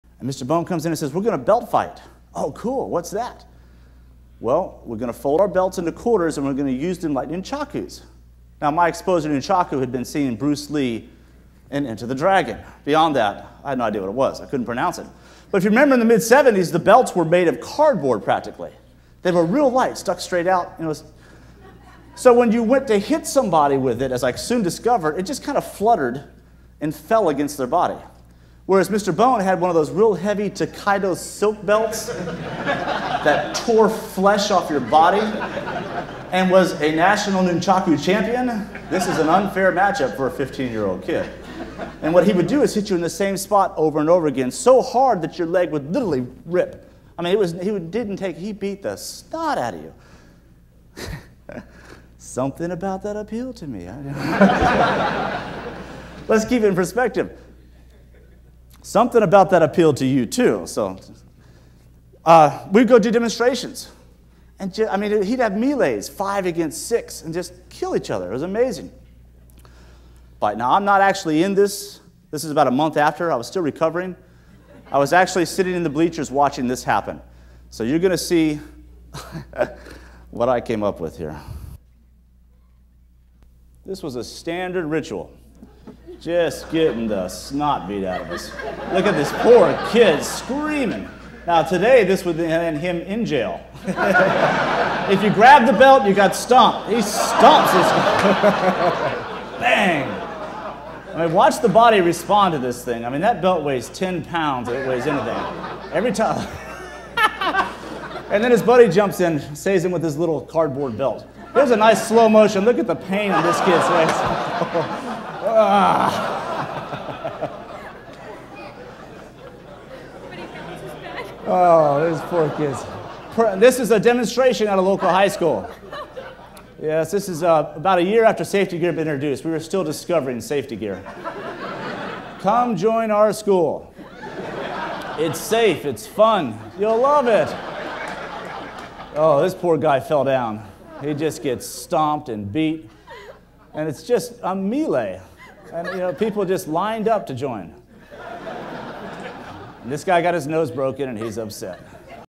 Karate Belt Fighting For Real (Crazy 1970s Blood and Guts Karate)
John Graden:
"Mr. Bone comes in and says, we're going to belt fight. Oh, cool. What's that? Well, we're going to fold our belts into quarters and we're going to use enlightening  nunchakus.

[00:00:08.490] - Speaker 1
Now, my exposure in nunchakus had been seen in Bruce Lee and Enter the Dragon. Beyond that, I had no idea what it was. I couldn't pronounce something, but if you remember, in the mid seventy s, the belts were made of cardboard, practically. They have a real light stuck straight out. So when you went to hit somebody with it, as I soon discovered, it just kind of fluttered and fell against their body.

[00:00:28.590] - Speaker 1
Whereas Mr. Bone had one of those real heavy Tokaido silk belts that poured flesh off your body and was a national nunchaku champion. This is an unfair matchup for a 15 year old kid. And what he would do is hit you in the same spot over and over again so hard that your leg would literally rip. He beat the snot out of you.

[00:00:52.370] - Speaker 1
Something about that appeal to me. Let's keep in perspective. Something about that appeal to you, too. We'd go do demonstrations. He'd have me laid five against six and just kill each other.

[00:01:08.380] - Speaker 1
It was amazing. I'm not actually in this. This is about a month after I was still recovering. I was actually sitting in the bleachers watching this happen. So you're going to see what I came up with here.

[00:01:23.750] - Speaker 1
This was a standard ritual, just getting the snot beat out of it. Look at this poor kid screaming now. Today, this is him in jail. If you grab the belt, you got stumped. He stumped.

[00:01:41.310] - Speaker 1
Watch the body respond to this thing. I mean, that belt weighs £10. It weighs anything.

[00:01:48.190] - Speaker 1
And then his buddy jumps and stays in with his little cardboard belt. There's a nice slow motion look at the pain of this kid, right?

[00:02:03.650] - Speaker 1
This is a demonstration at a local high school. Yes. This is about a year after safety gear was introduced. We were still discovering safety gear. Come join our school.

[00:02:15.110] - Speaker 1
It's safe, it's fun, you'll love it. Oh, this poor guy fell down. He just gets stomped and beat, and it's just a melee. And it's people just lined up to join. This guy got his nose broken and he's upset."